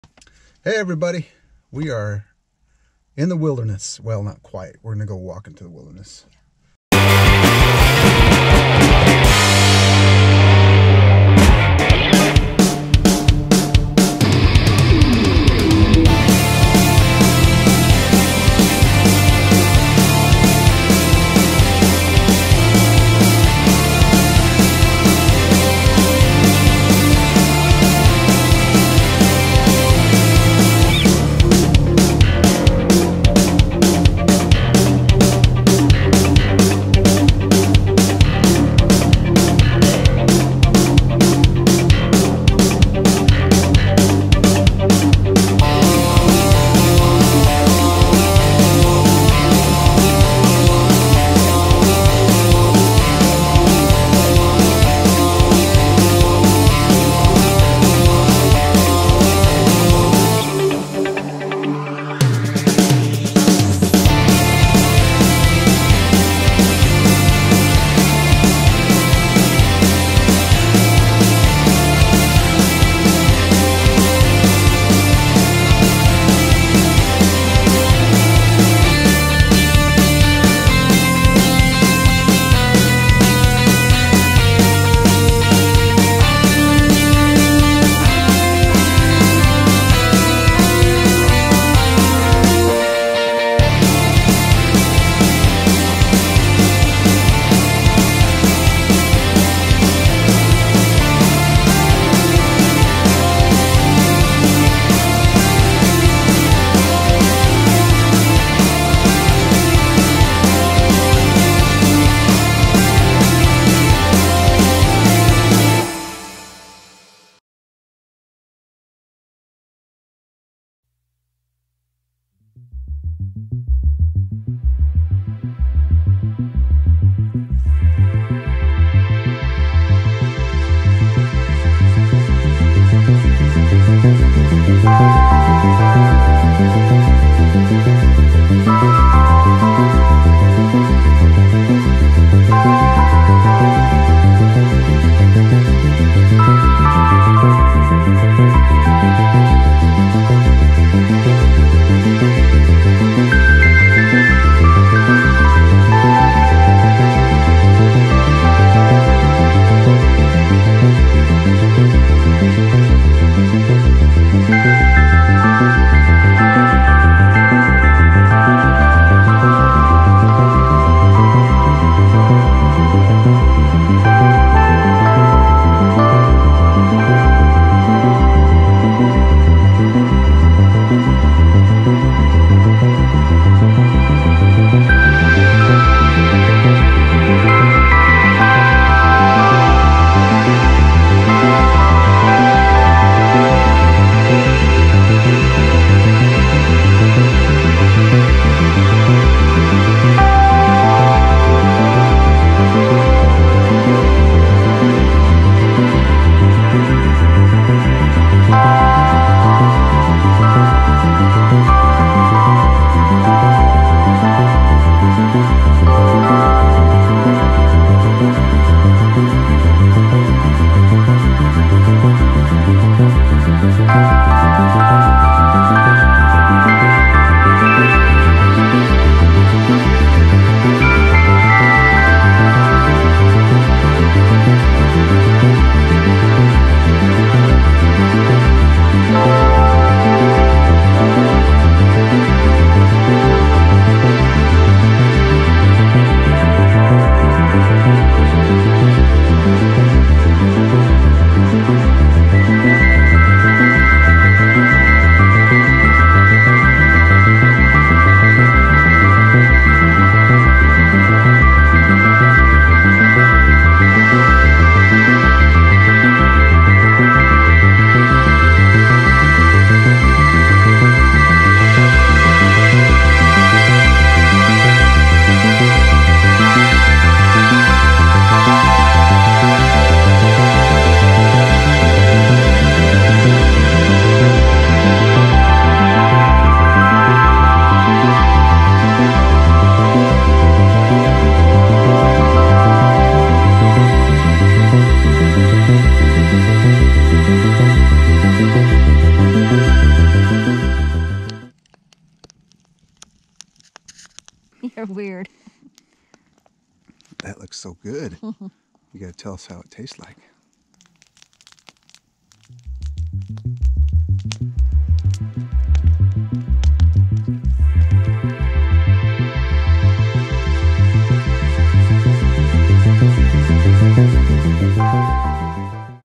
Walking the Wilderness (social distancing)
A walk on the wild side. We went to the juniper Forest and saw lots of signs of wildlife.